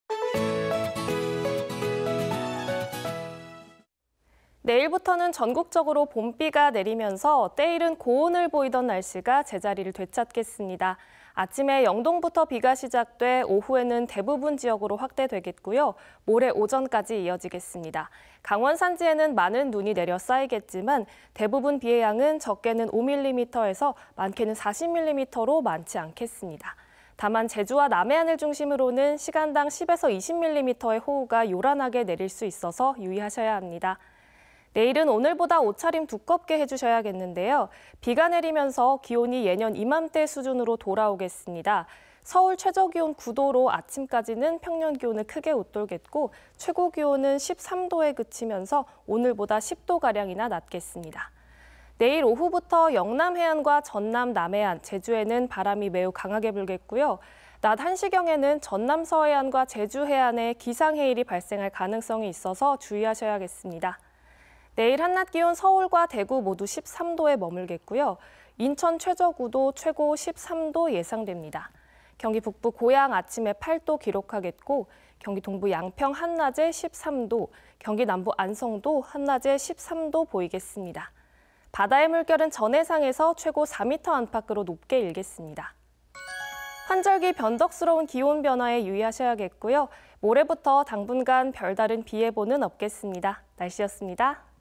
[3월 25일 날씨] 봄비에 고온 현상 주춤...낮 10도 가량 내려가요 (문지영 기상캐스터)
내일부터는 전국적으로 봄비가 내리면서 때이른 고온을 보이던 날씨가 제자리를 되찾겠습니다.
아침에 영동부터 비가 시작돼 오후에는 대부분 지역으로 확대되겠고요. 모레 오전까지 이어지겠습니다. 강원산지에는 많은 눈이 내리겠지만, 대부분 비의 양은 적게는 5mm에서 많게는 40mm로 많지는 않겠습니다. 다만 제주와 남해안을 중심으로는 시간당 10~20mm의 호우가 요란하게 내릴 수 있어 유의하셔야합니다.
내일은 오늘보다 옷차림 두껍게 해주셔야겠는데요. 비가 내리면서 기온이 예년 이맘때 수준으로 돌아오겠습니다. 서울 최저기온 9도로 아침까지는 평년기온을 크게 웃돌겠고, 최고기온은 13도에 그치면서 오늘보다 10도가량 낮겠습니다.
내일 오후부터 영남해안과 전남남해안, 제주에는 바람이 매우 강하게 불겠고요. 낮 한시경에는 전남서해안과 제주해안에 기상해일이 발생할 가능성이 있어 주의하셔야겠습니다.
내일 한낮기온 서울과 대구 모두 13도에 머물겠습니다. 인천 최저 9도, 최고 13도 예상되고, 경기북부 고양 아침에 8도 기록하겠습니다. 경기동부 양평 한낮 13도, 경기남부 안성도 한낮에 13도 보이겠습니다.
바다의 물결은 전 해상에서 최고 4m 안팎으로
높게 일겠습니다.
환절기 변덕스러운 기온 변화에 유의하셔야겠고요. 모레부터 당분간 별다른 비 예보는 없겠습니다. 날씨였습니다.